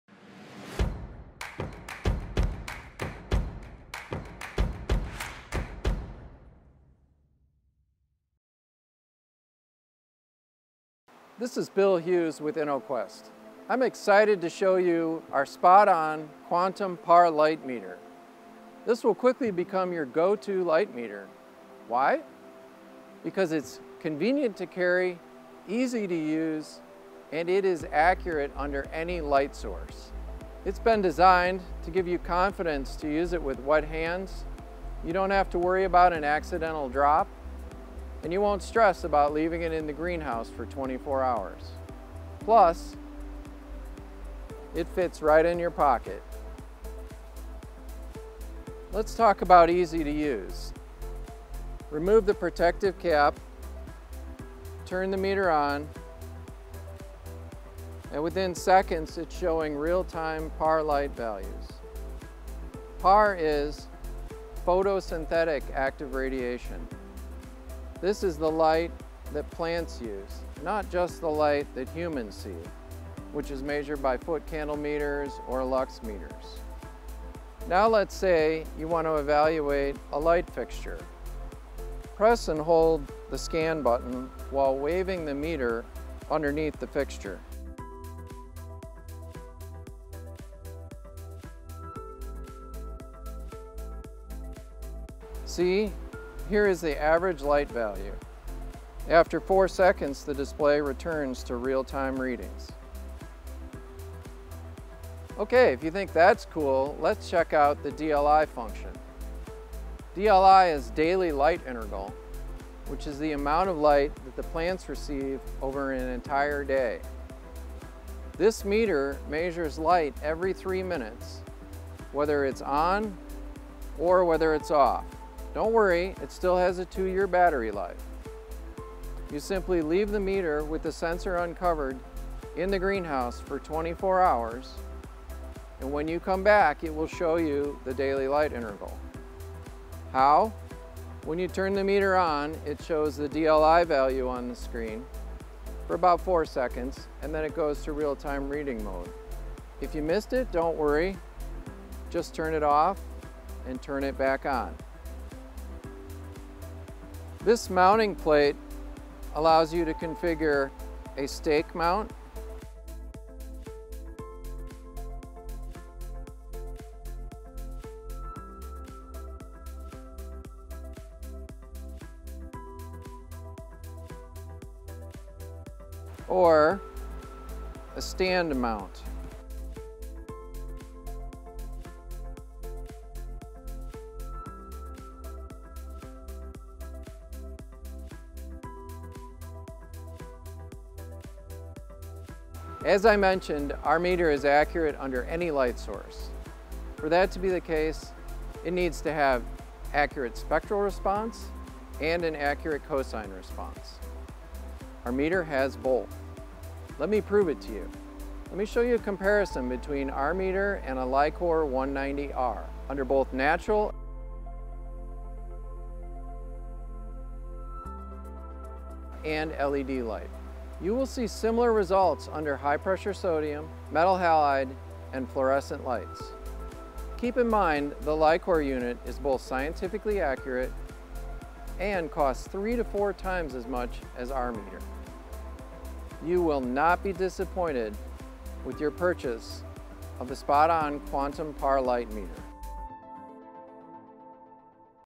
SpotOn® Quantum PAR Light Meter
The SpotOn® Quantum PAR Light Meter is an affordable meter that provides scientifically accurate Photosynthetic Active Radiation (PAR) light readings under any light source including LED!